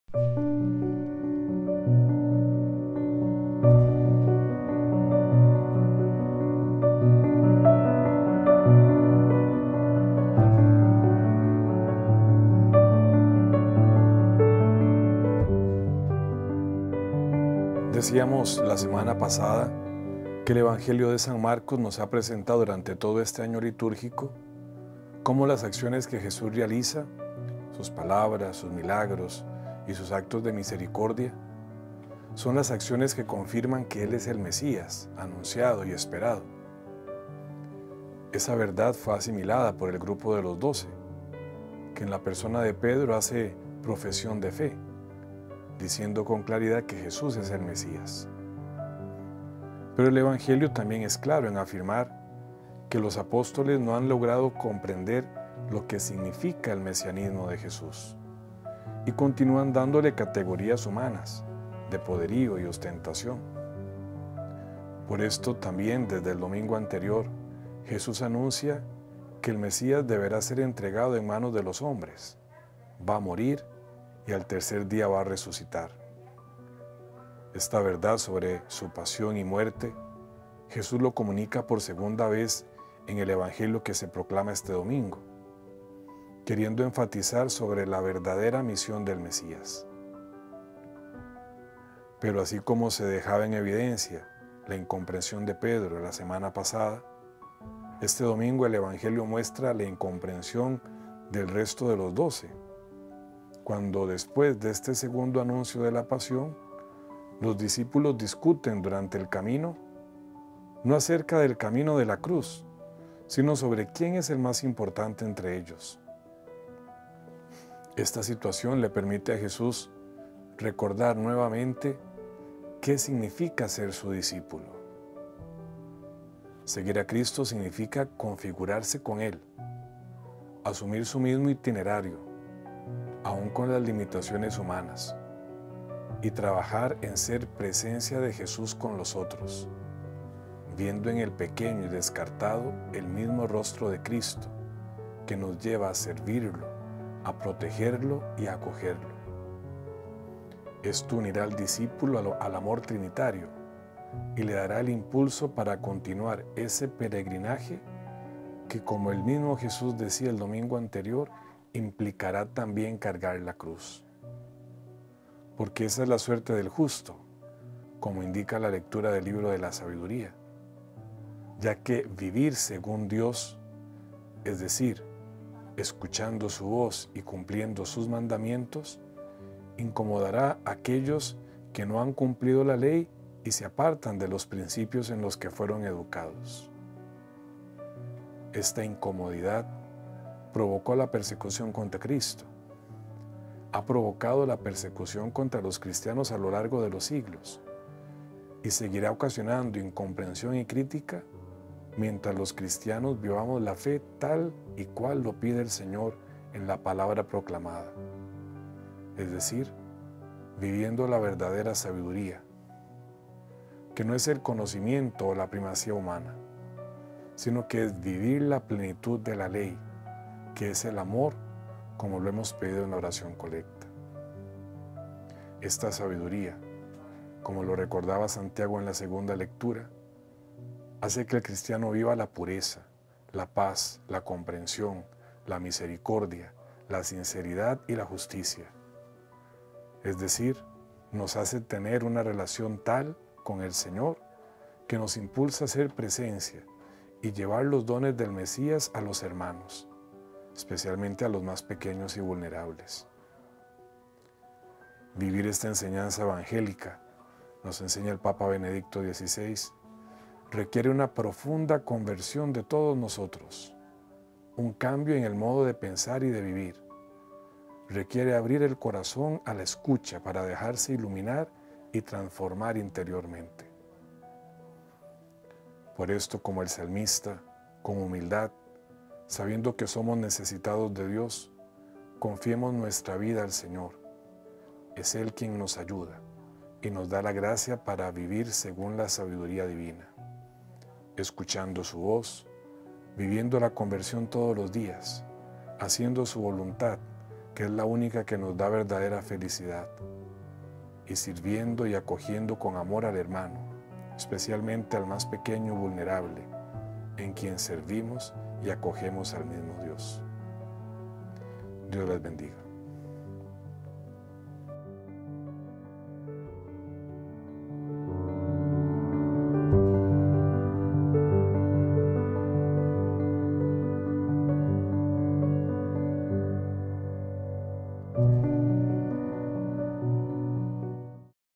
XXV Domingo del Tiempo Ordinario - Mensaje Monseñor Daniel Blanco
ObispoAuxiliar | XXV Domingo del Tiempo Ordinario, Mons. Daniel Francisco Blanco Méndez Obispo Auxiliar de San José – Costa Rica.